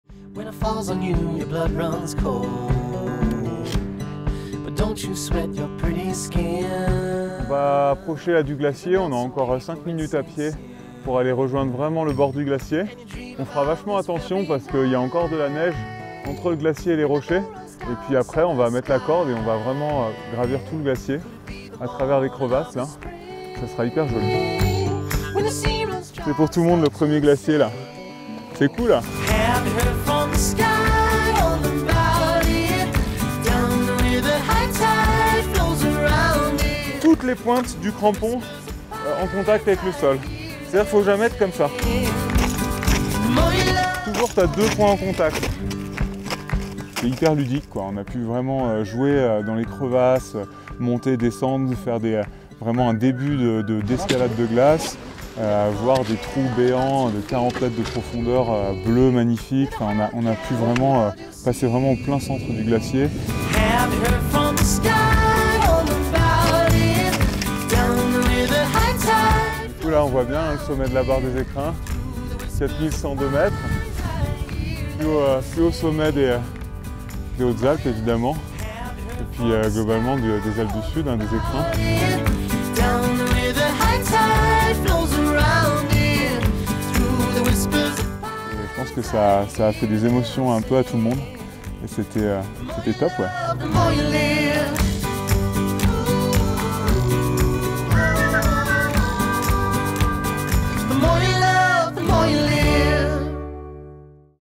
Prenez de la hauteur dans le Pays des Écrins
Évoluer sur la glace tout en approchant les crevasses, découvrir un glacier, crampons aux pieds... Les guides du Pays des Écrins vous invitent à vivre une expérience inoubliable, unique et très accessible de la marche sur glacier ! ❄️🏔

📍 Glacier Blanc, Parc national des Ecrins
🎥  Boris Doye Films